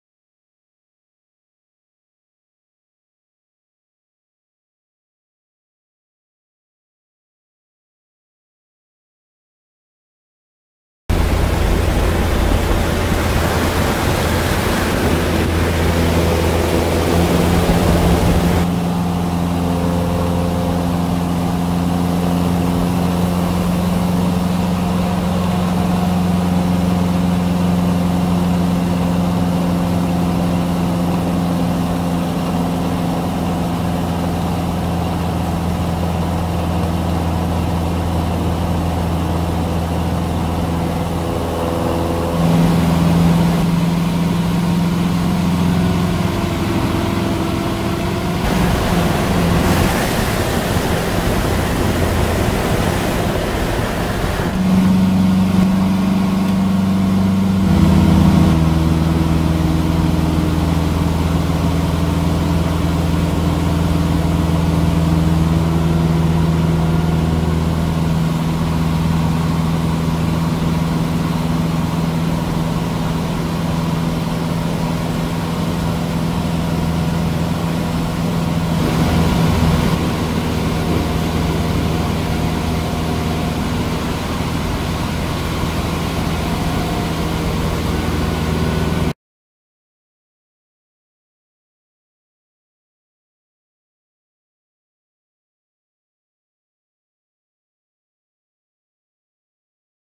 Ribbon Cutting
Ribbon Cutting with a Cessna 150.  Drop cheapest biodegradable TP.  Make a Williamson Turn using 45 degree steep turns and descend 1,000ft while doing so.  Ribbon will be right in front of you.